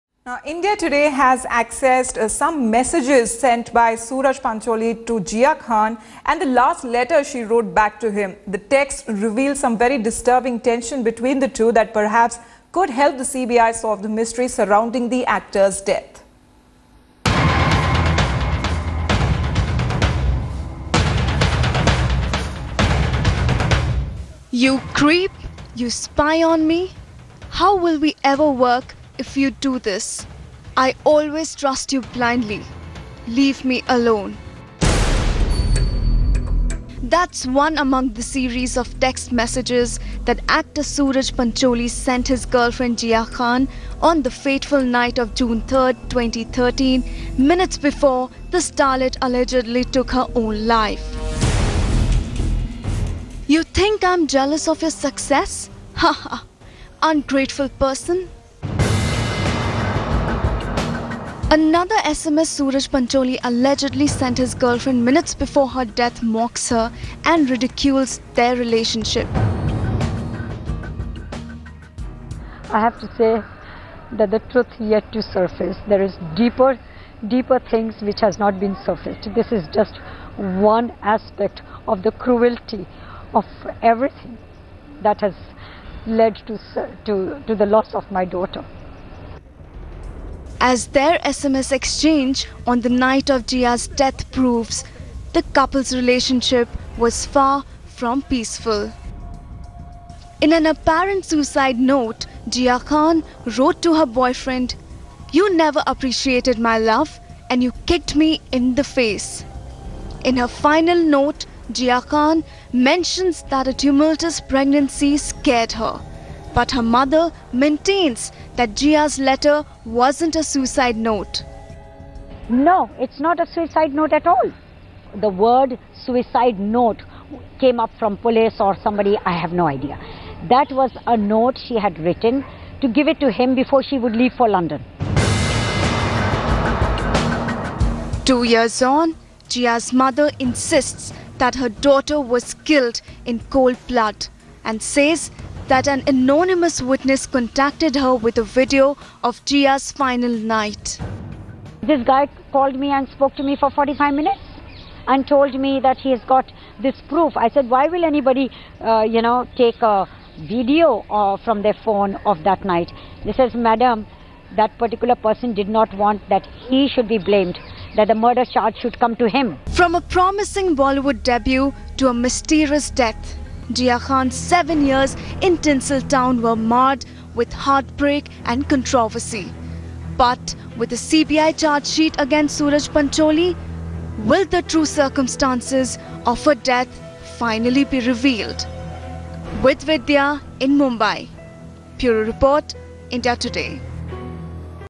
JiahMystery: Text Messages Revealed
Bollywood actor Jiah Khan's murder mystery took a new turn after the CBI filed it's chargesheet.